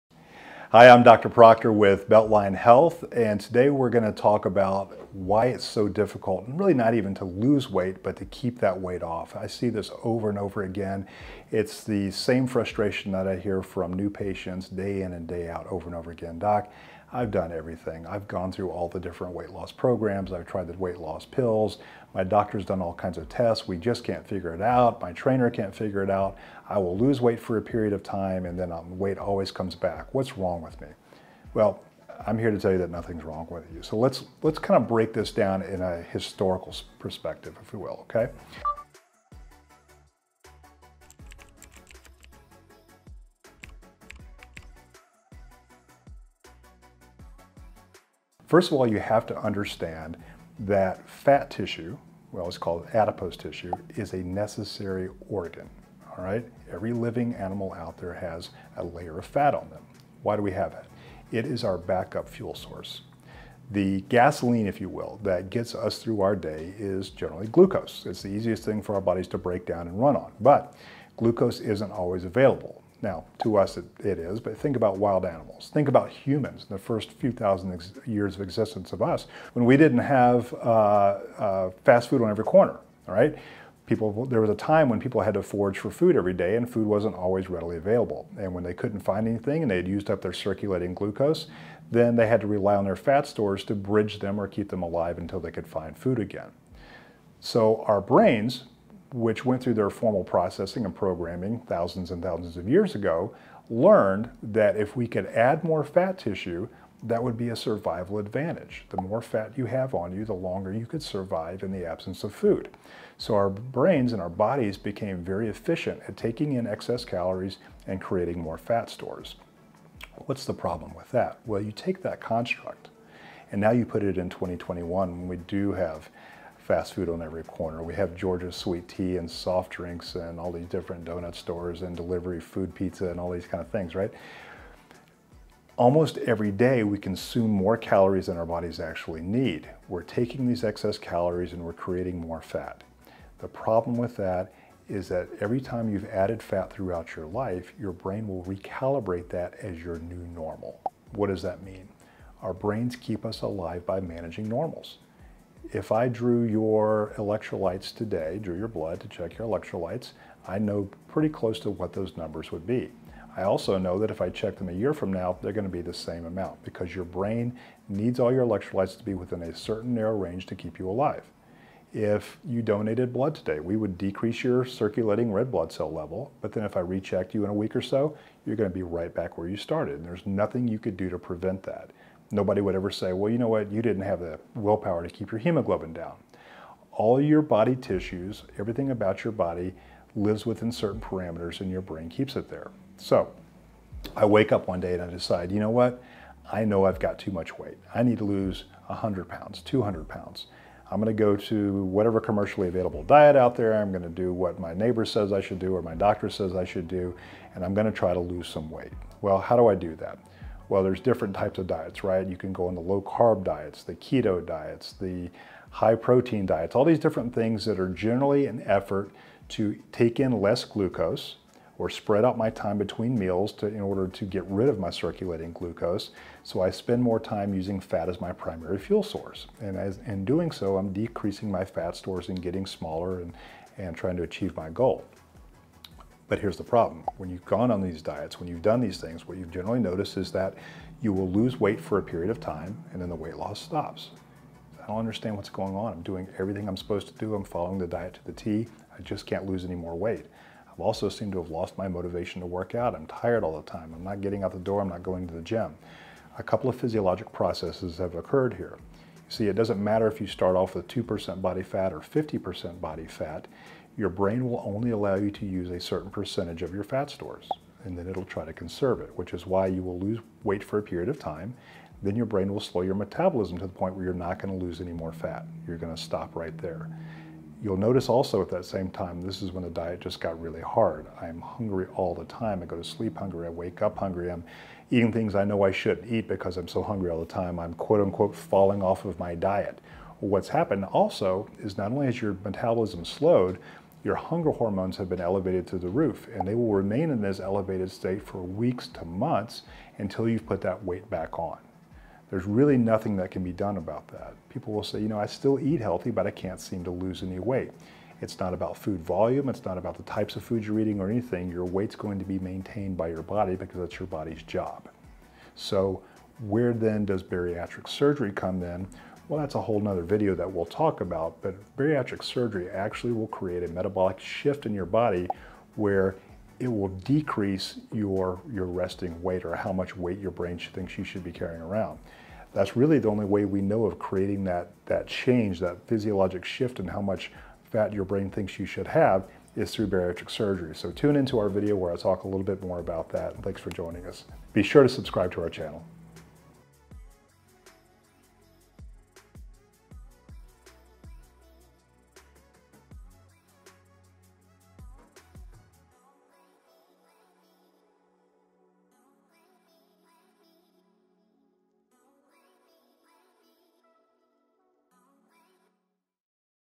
Why Can't I Lose Weight? | Reasons Why Weight Loss is Difficult
Thousands of years ago, our bodies were configured to hold on to calories in the form of fat to avoid starvation. This has made it difficult for us to lose weight and keep it off due to the metabolic and hormonal shifts that occur in our bodies.

In this video, Dr. Charles Procter at Beltline Health, details the science and the mechanisms behind fat storage and why it can be so hard to lose weight, plus he talks about the reason bariatric surgery is so beneficial in helping patients lose weight and maintain that weight loss long term.